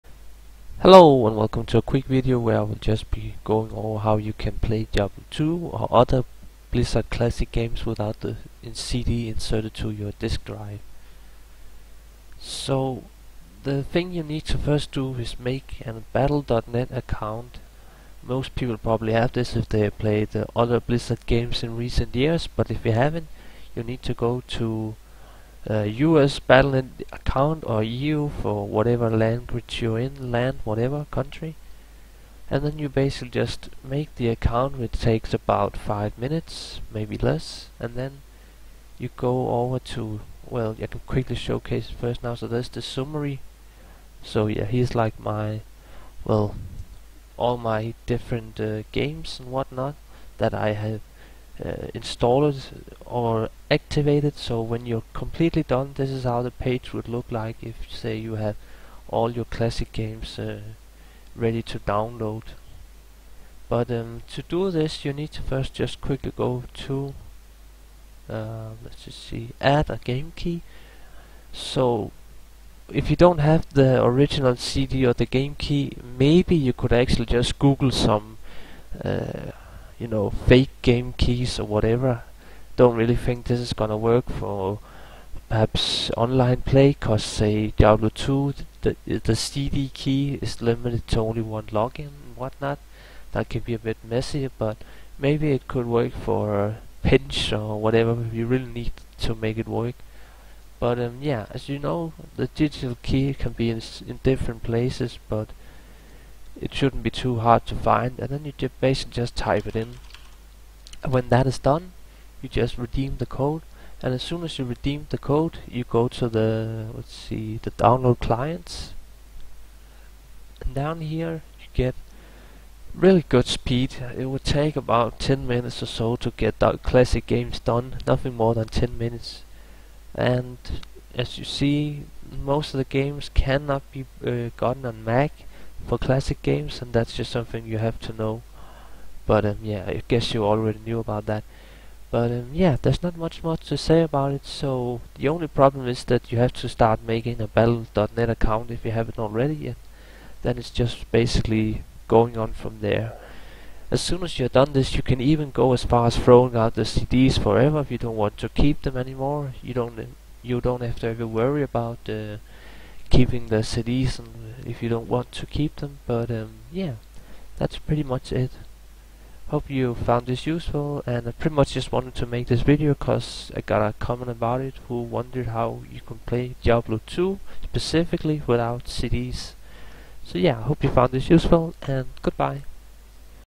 How to play Classic Blizzard games without CD
Quick tutorial on how to play classic blizzard games without the CD. You need CD key only to make this work.